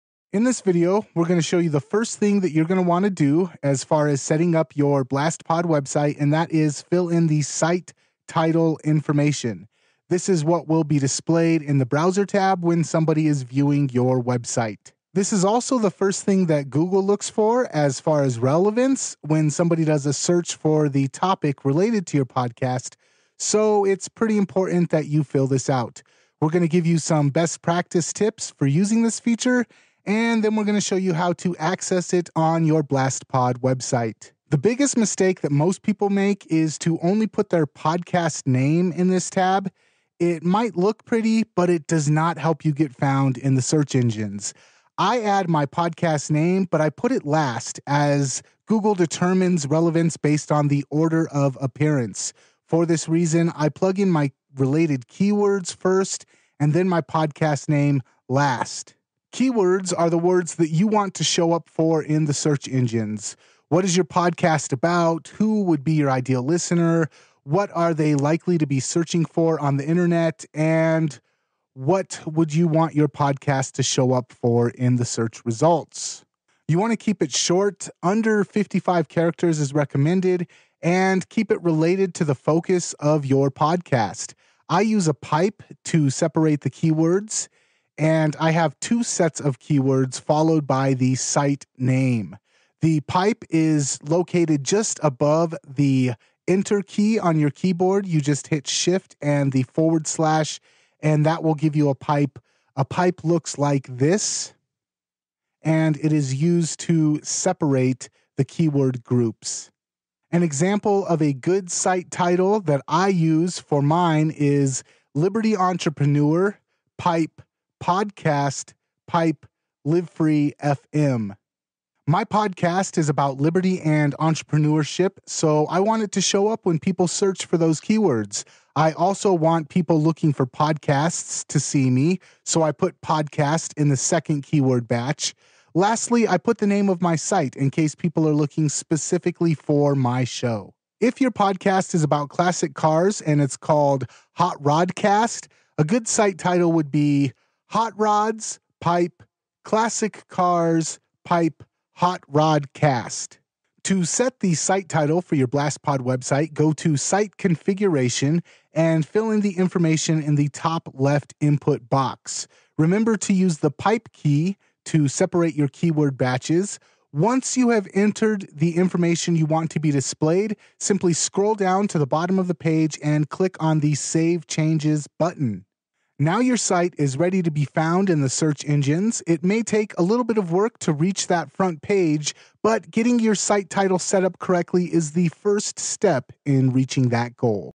Changing your Blastpod Site Title
How to use BlastPod to set your podcast website title. Tips on how to make sure your podcast is search engine friendly.